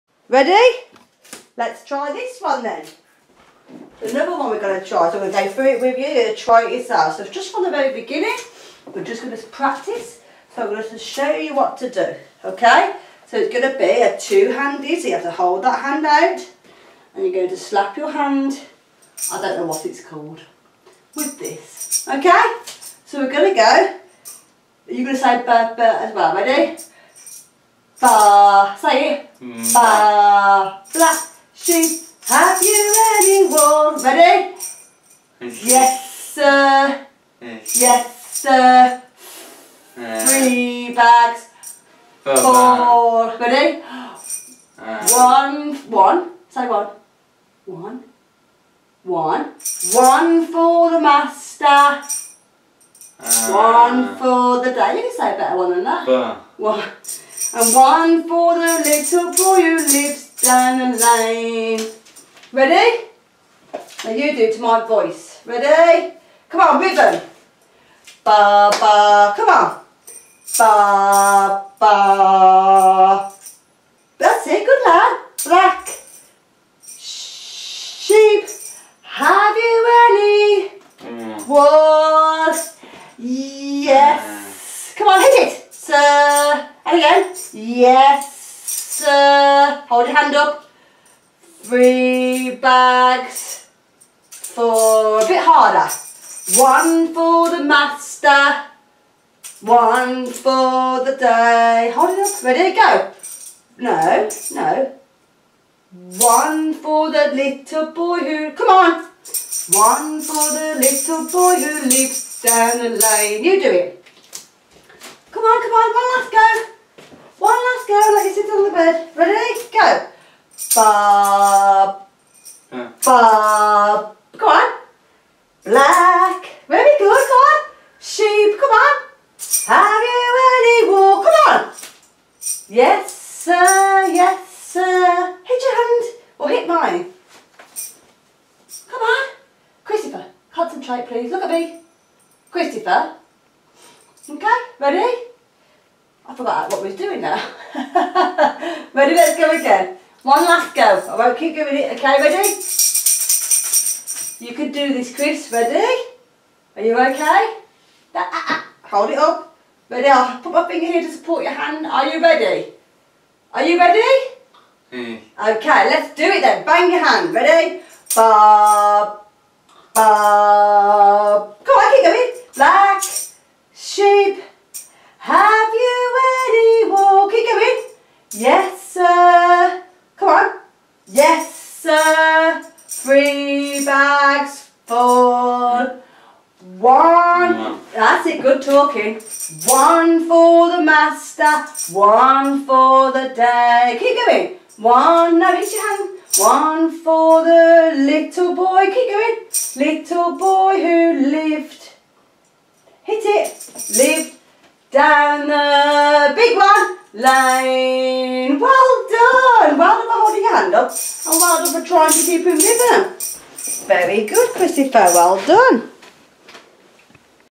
28.9.22 Dawn skill work - using 2 hands
I asked Dawn to help Chris learn to use 2 hands - she was using a hand tambourine and also working on his speech   ( he still loves hearing nursery rhymes!)